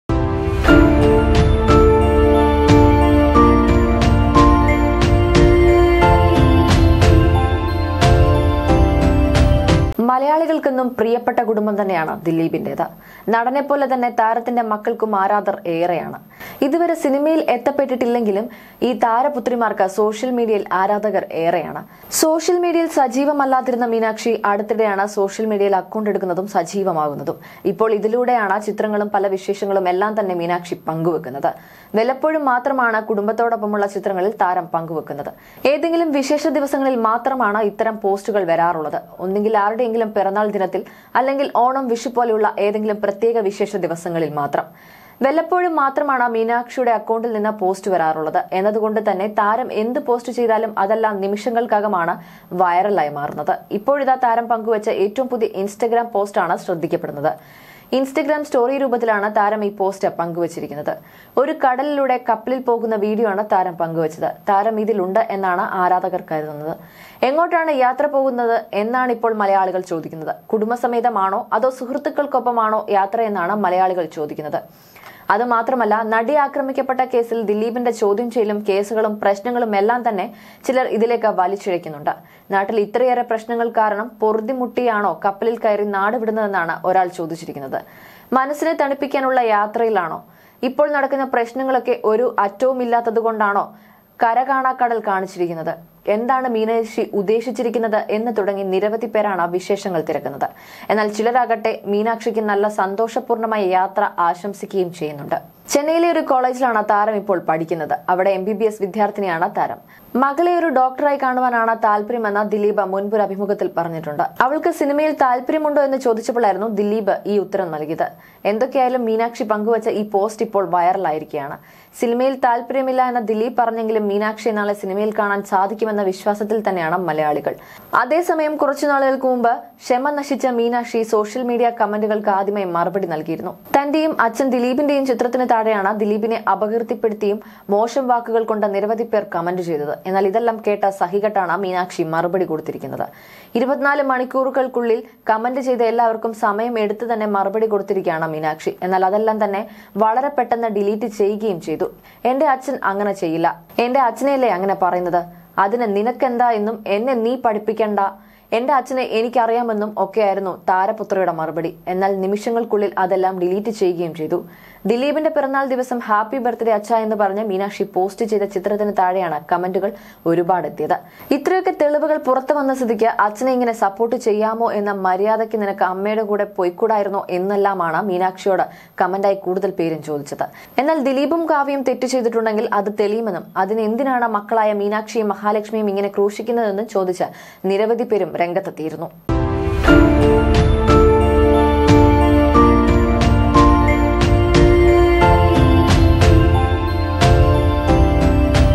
Dileep | കരകാണാ കടലില്‍ മീനാക്ഷി
ചെന്നൈയിലെ ഒരു കോളേജില്‍ ആണ് താരം ഇപ്പോള്‍ പഠിക്കുന്നത്. അവിടെ എംബിബിഎസ് വിദ്യാര്‍ഥിനി ആണ് താരം. മക്കളെ ഒരു ഡോക്ടര്‍ ആയി കാണുവാന്‍ ആണ് താല്പര്യം എന്ന് ദിലീപ് മുന്‍പ് ഒരു അഭിമുഖത്തില്‍ പറഞ്ഞിട്ടുണ്ട്. അവള്‍ക്ക് സിനിമയില്‍ താല്പര്യം ഉണ്ടോ എന്ന് ചോദിച്ചപ്പോള്‍ ആയിരുന്നു ദിലീപ് ഈ ഉത്തരം നല്‍കിയത്. എന്തൊക്കെയായാലും മീനാക്ഷി പങ്കുവെച്ച ഈ പോസ്റ്റ് ഇപ്പോള്‍ വൈറലായിരിക്കുകയാണ്... @MovieBrand794